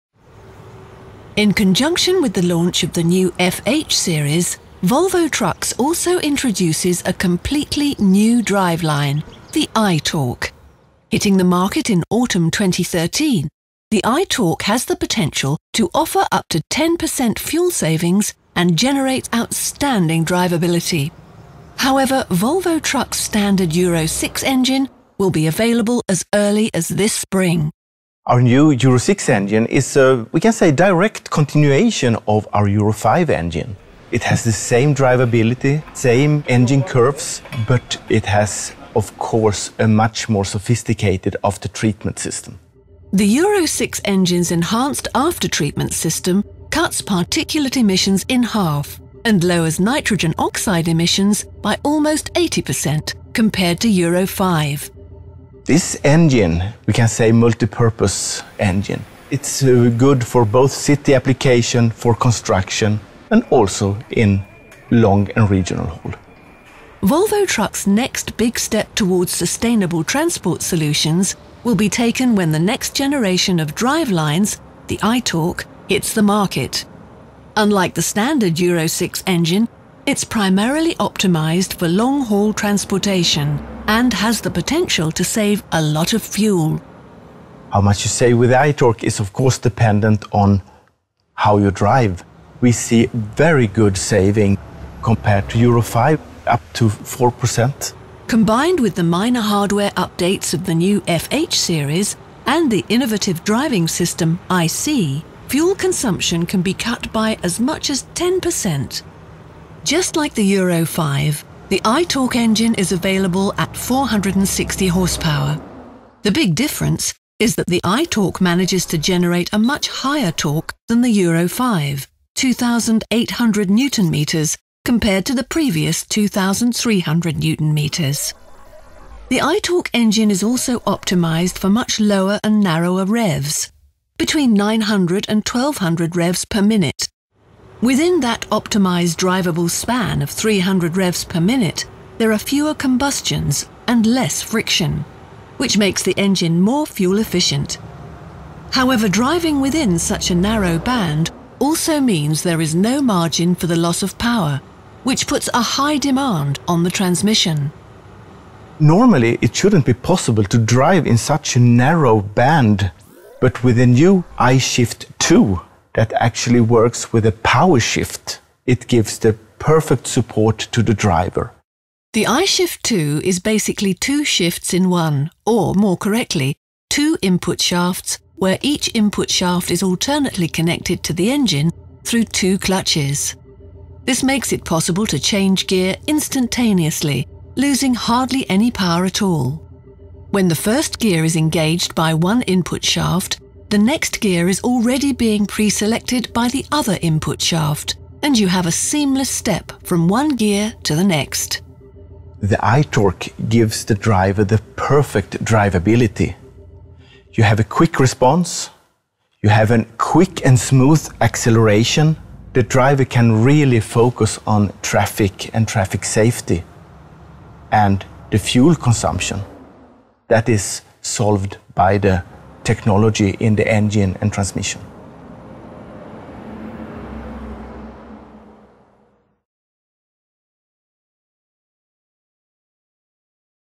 Le nouveau Volvo FH : la chaîne cinématique
Volvo Trucks est leader en matière de développement de chaînes cinématiques destinées aux véhicules industriels. Le nouveau système I-Torque de Volvo est conforme à la norme Euro 6 et offre une motricité exceptionnelle.
Associé à l'I-See, il permet également de réduire jusqu'à 10 % la consommation de carburant.